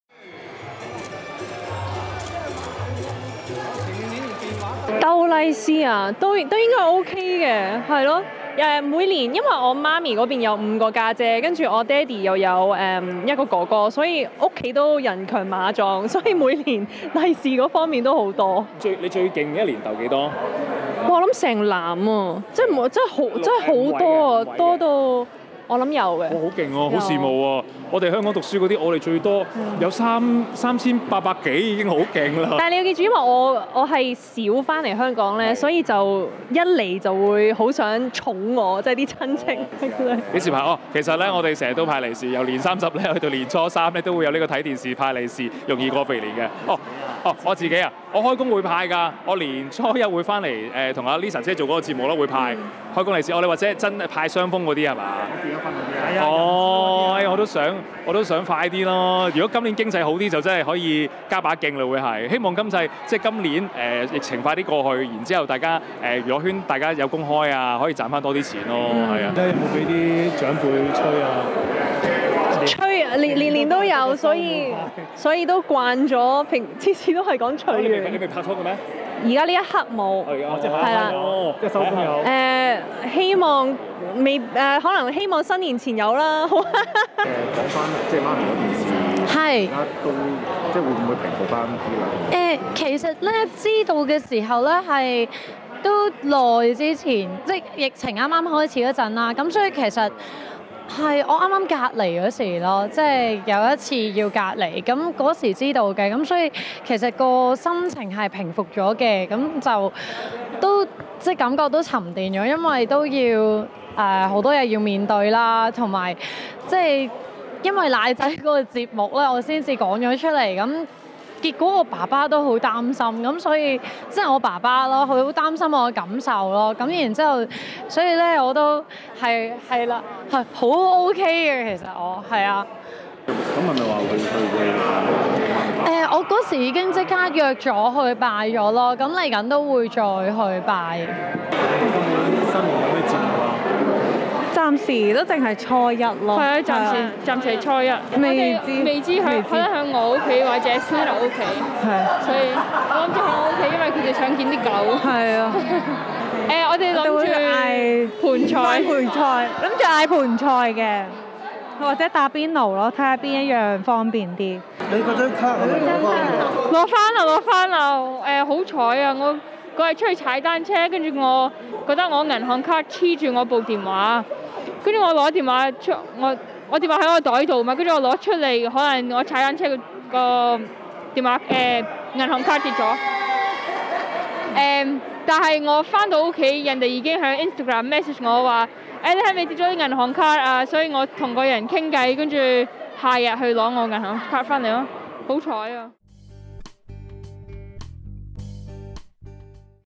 鄧佩儀每年收5位數利市錢 陸浩明聽到好羨慕 (2021.02.10)
陸浩明、鄧佩儀、孫慧雪、應屆港姐冠軍謝嘉怡、亞軍陳楨怡及季軍郭柏妍等，今日出席遊戲《睇電視𢭃利是融易過肥年》記者會。陸浩明被問到何時結婚派利市時說︰「我也想快點，如果今年經濟好轉會加把勁。」鄧佩儀習慣每逢新年被催婚，但她現在無拖拍，希望新年前遇到。她自爆每年可𢭃到金額達5位數的利市錢，令陸浩明非常羨慕。

孫慧雪提到失散20年的母親早已離世一事時說︰「是去年的事，當時的確有點心情沉重，但現在已平復。爸爸有擔心我的感受，我現已整頓過來，不想家人擔心，事後亦已去拜祭媽咪。」

陳楨怡及郭柏妍已約好新年去謝嘉怡屋企吃盆菜或打邊爐。謝嘉怡雖然有個廚師爸爸，但她自嘲廚藝一般。」

MingPao Ol